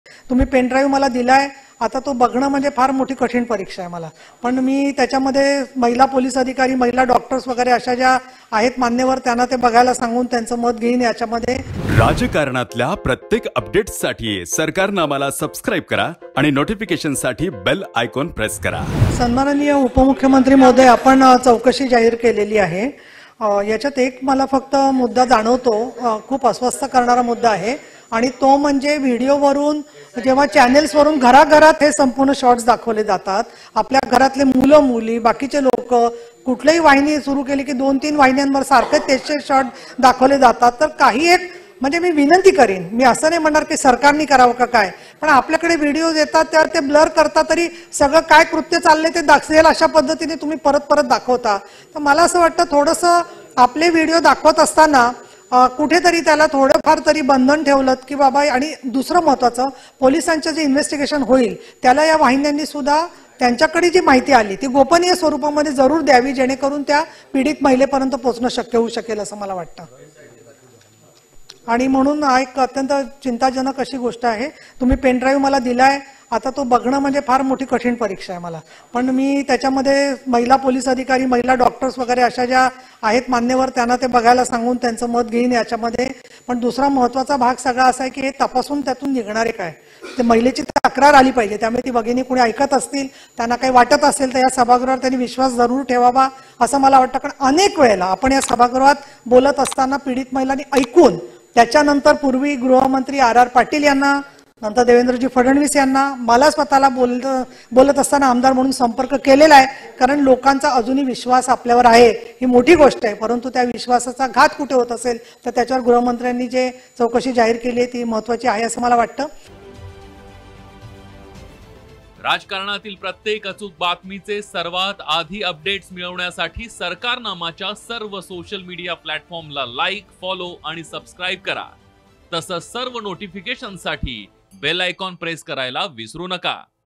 Kirit Somaiya viral video ;डॉक्टर, महिला पोलिसांना घेऊन 'तो' पेनड्राइव्ह बघणार' । Neelam Gorhe
Kirit Somaiya Viral Video Pendrive News - डॉक्टर, महिला पोलिसांना घेऊन 'तो' पेनड्राइव्ह बघणार' । Neelam Gorhe। Vidhan Parishad। Devendra Fadnavis।

(KB0723)
(NS0723)